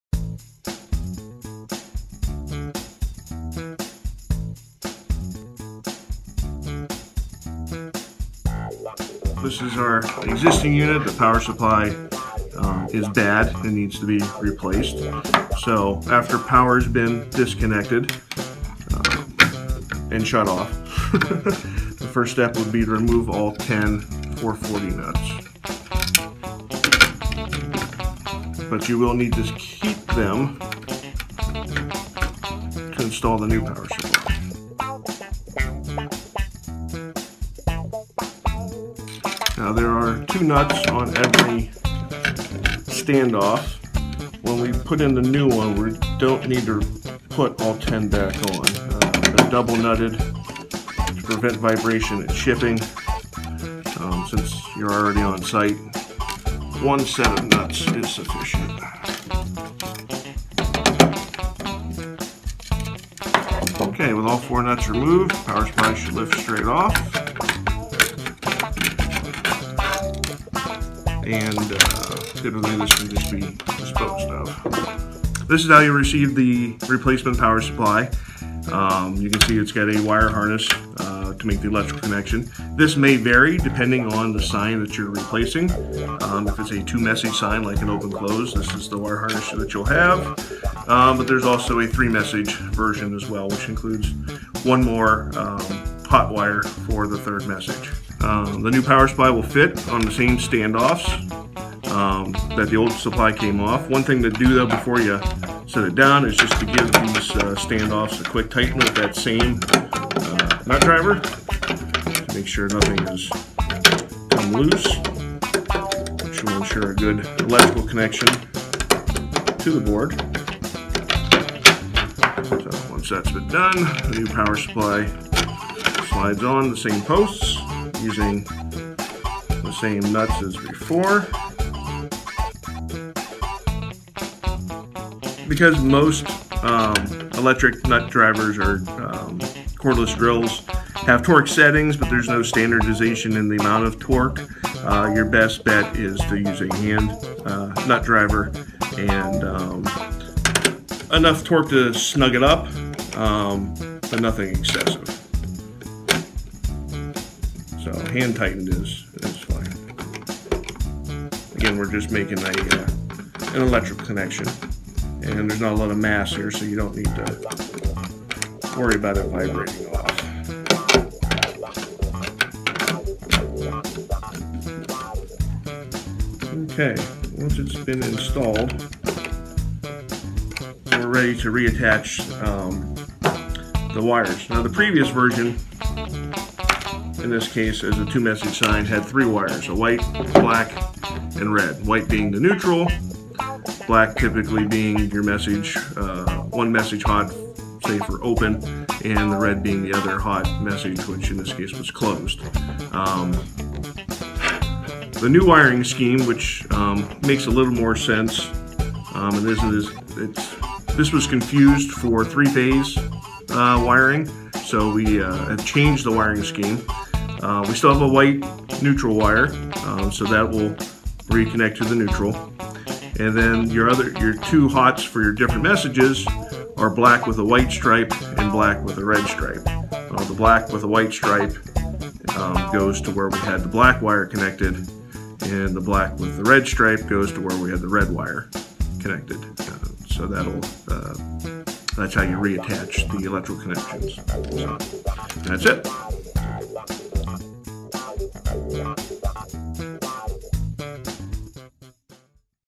Replacing An Old Power Supply With A New One
Step-by-step "how to" video demonstrating how to replace an old power supply with a new power supply.